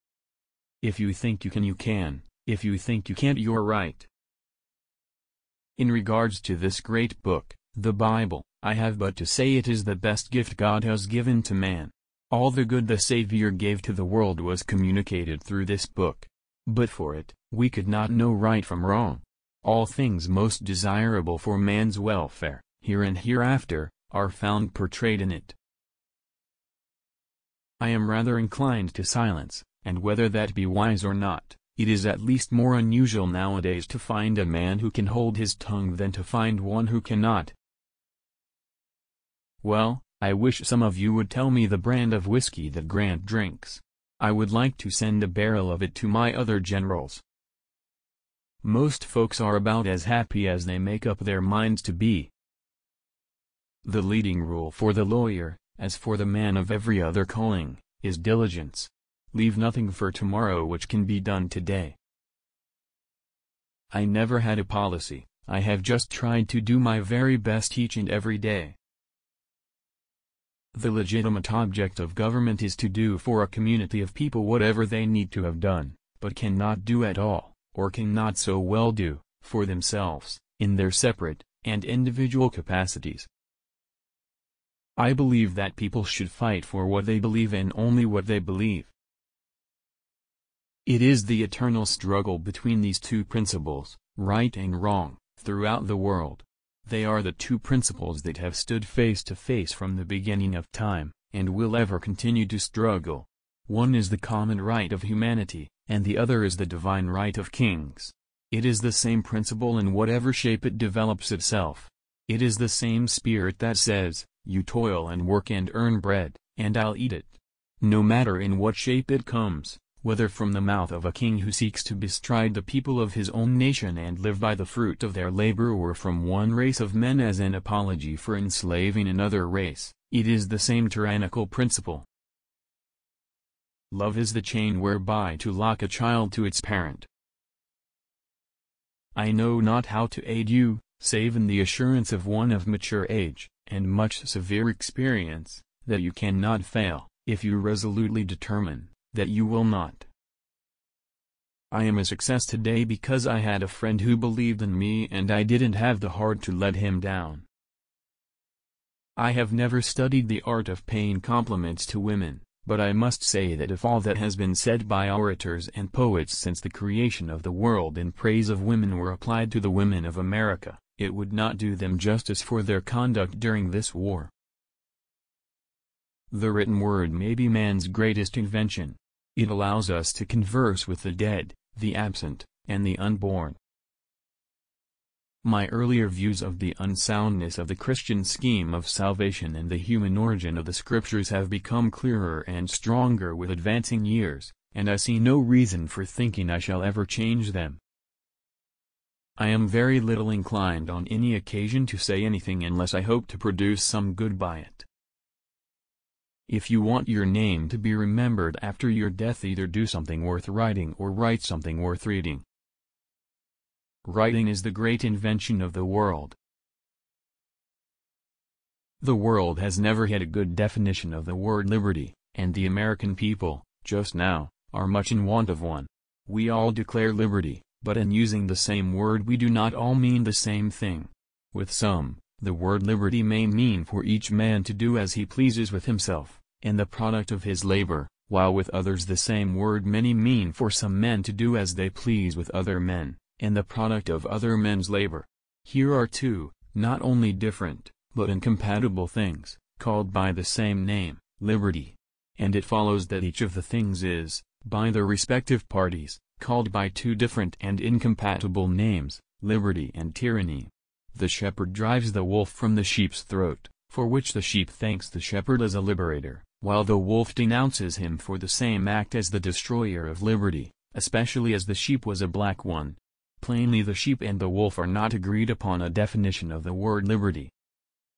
Abraham Lincoln Quotes
Abraham Lincoln (February 12, 1809 – April 15, 1865) was an American lawyer and statesman who served as the 16th president of the United States from 1861 until his assassination in 1865.